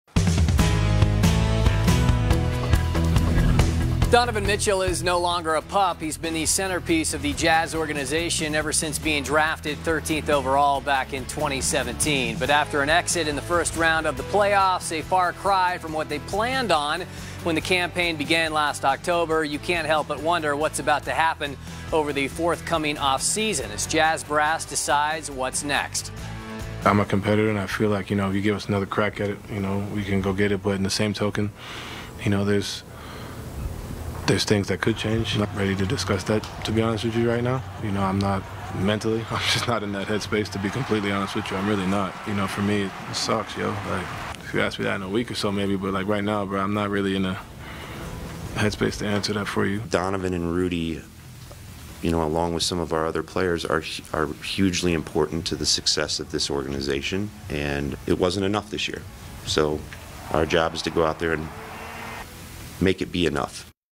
What's next for the Utah Jazz after first-round playoff elimination?
Donovan Mitchell and Jazz general manager Justin Zanik talk to reporters after last night's heartbreaking loss.